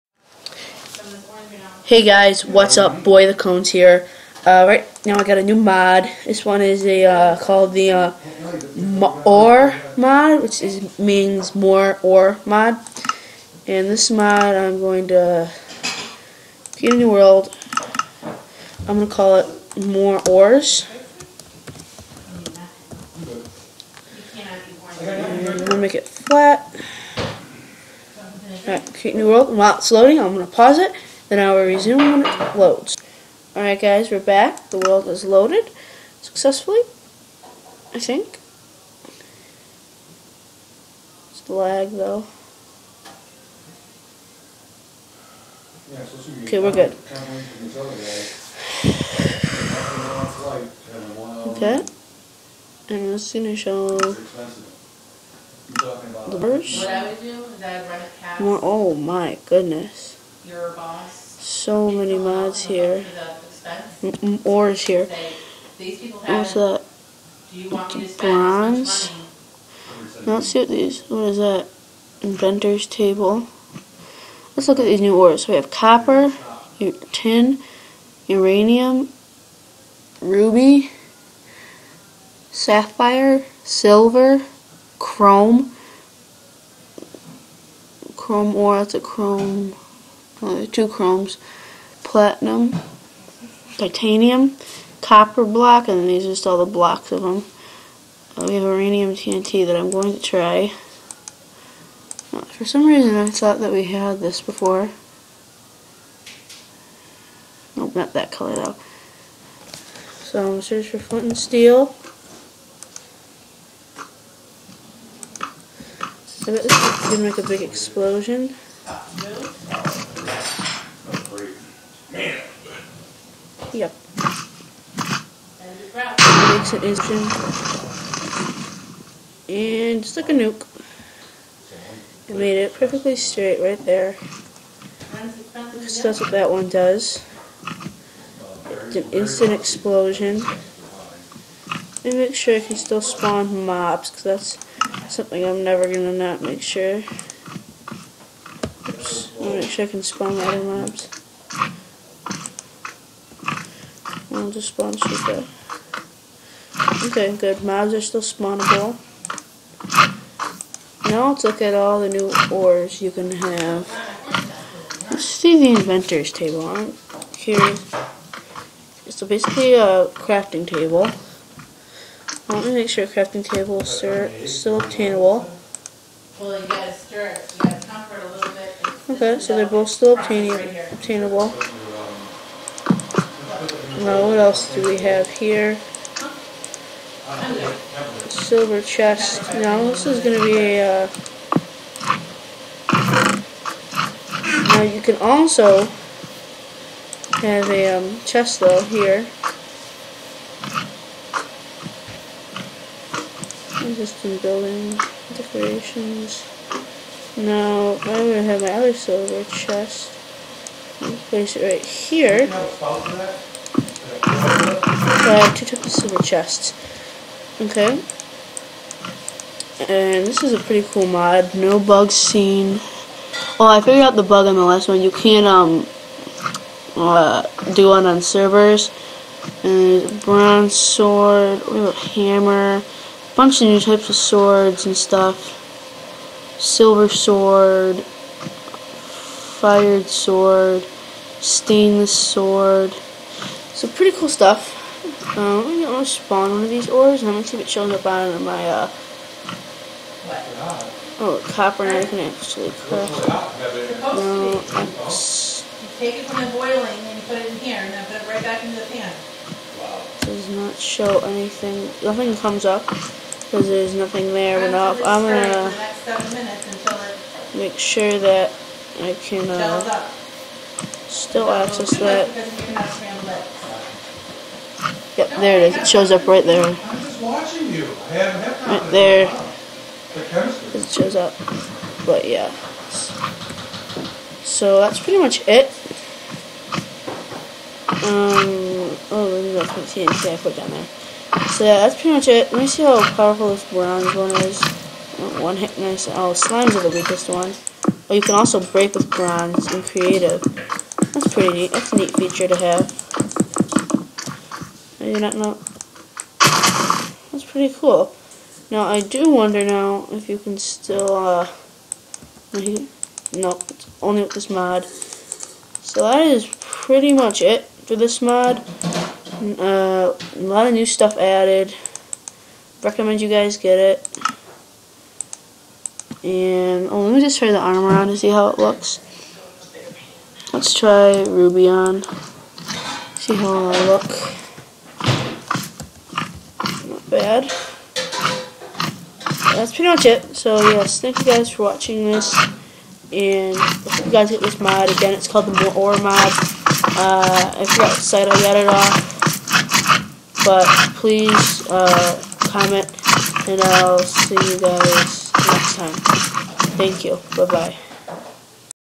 More ores mod
This is me playing the more ores mod.